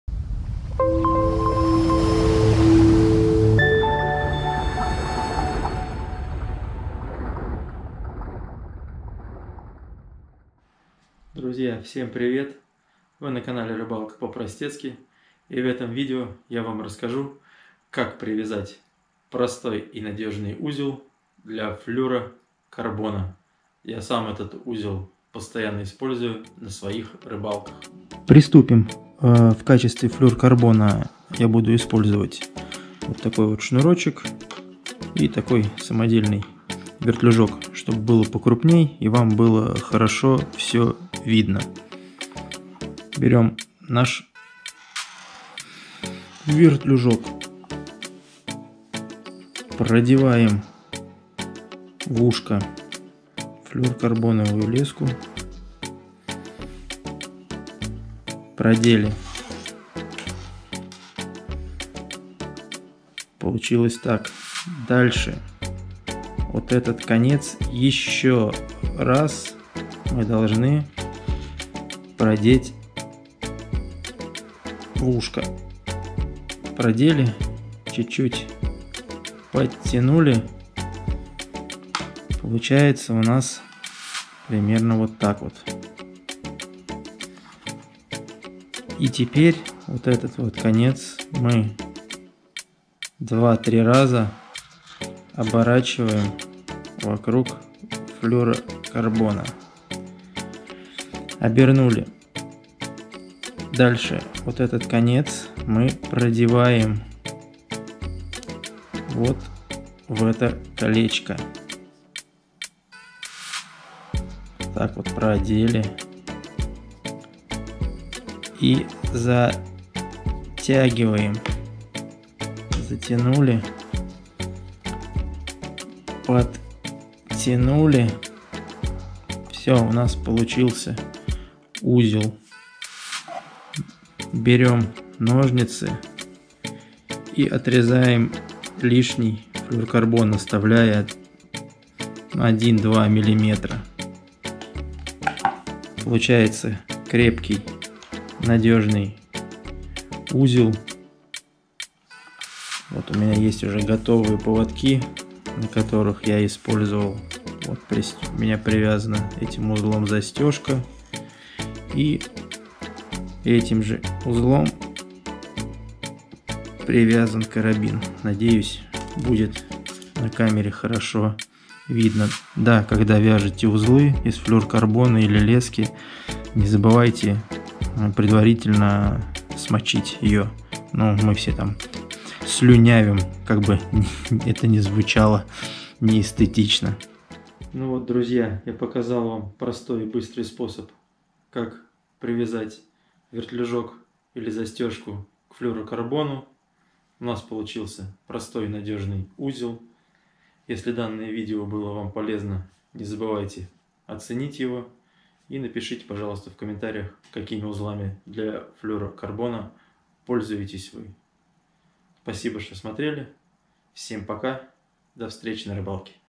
Простой,надежный Узел для Флюорокарбона Как привязать Флюр?
Приветствую вас на канале Рыбалка по-простецки
Как привязать флюорокарбон к крючку, застежке или вертлюжку?
Сегодня мы рассмотрим узел, которым мы привязываем флюорокарбоновый поводок к застежке или к вертлюгу.
Я покажу, как его связать и затянуть, данный узел для флюорокарбона крепче, чем другие узлы, которые вы знаете.
Узел универсален, и может использоваться не только с флюорокарбоном, но и с обычными монофильными лесками, и не только в джиге...
Данный узел для флюорокарбоновых лесок для меня лучший.